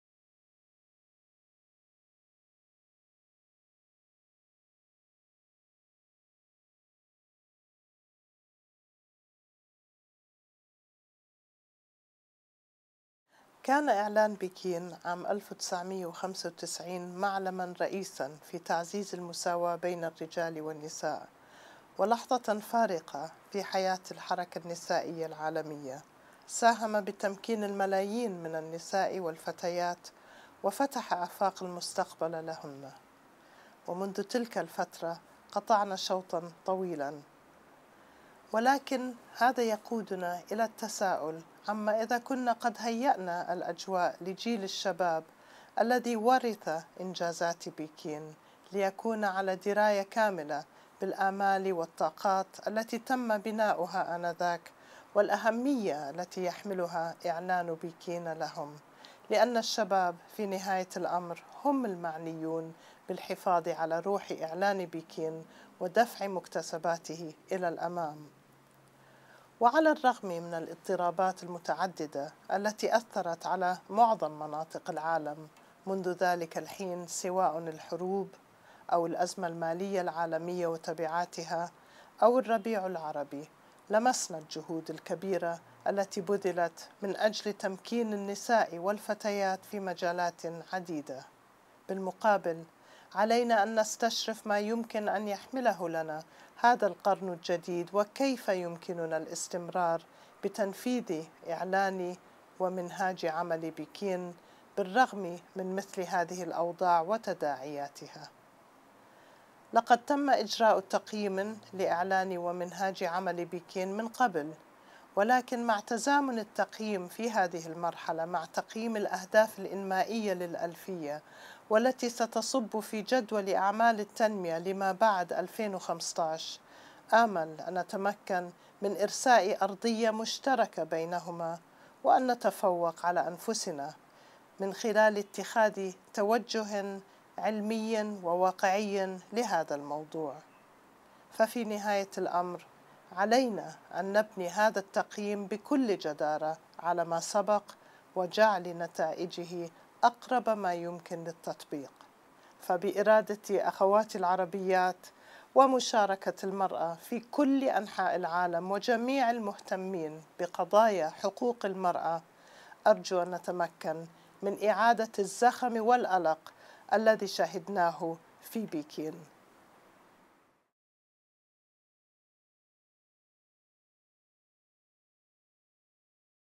رسالة من صاحبة السمو الملكي الأميرة بسمة بنت طلال
رسالة لمزيد من الأمل ونداء لتفعيل دور الشباب أطلقتهما صاحبة السمو الملكي الأميرة بسمة بنت طلال بمناسبة الإحتفال بإنطلاق عملية مراجعة وتقييم إعلان بكين في الأردن.